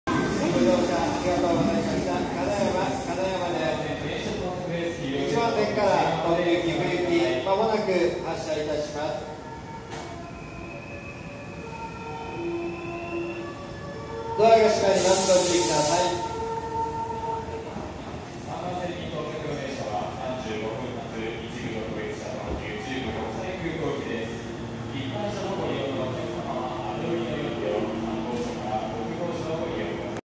「到着MH！」名鉄2200系2205f金山到着MHフル。
7月28日撮影。
極小で後半だけしか聞こえないですが鳴りました！
まさか鳴るとは思ってなかったw。